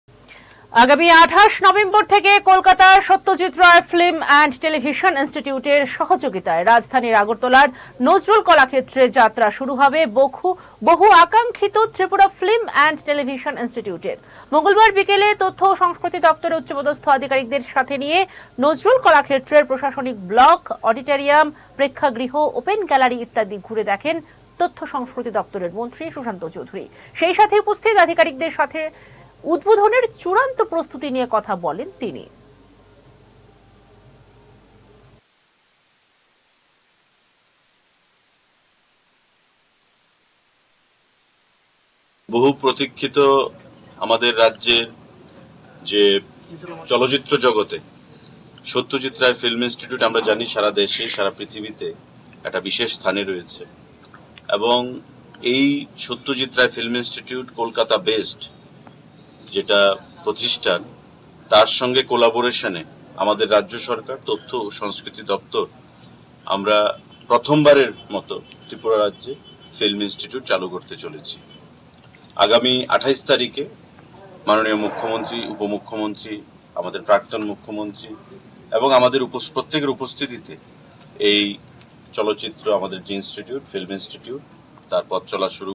চূড়ান্ত প্রস্তুতি খতিয়ে দেখেন তথ্যমন্ত্রী সুশান্ত চৌধুরী
আগামী ২৮ নভেম্বর থেকে কলকাতার সত্যজিৎ রায় ফিল্ম এন্ড টেলিভিশন ইন্সটিটিউটের সহযোগীতায় রাজধানী আগরতলার নজরুল কলাক্ষেত্রে যাত্রা শুরু হবে বহু আকাঙ্ক্ষিত ত্রিপুরা ফিল্ম এন্ড টেলিভিশন ইন্সটিটিউটের। মঙ্গলবার বিকেলে তথ্য ও সংস্কৃতি দপ্তরের উচ্চপদস্থ আধিকারিকদের সাথে নিয়ে নজরুল কলাক্ষেত্রের প্রশাসনিক ব্লক, অডিটরিয়াম, প্রেক্ষাগৃহ, ওপেন গ্যালারী ইত্যাদি ঘুরে দেখেন তথ্য সংস্কৃতি মন্ত্রী সুশান্ত চৌধুরী। সেই সাথে উপস্থিত আধিকারিকদের সঙ্গে সাথে উদ্বোধনের চূড়ান্ত প্রস্তুতি নিয়ে কথা বলেন তিনি।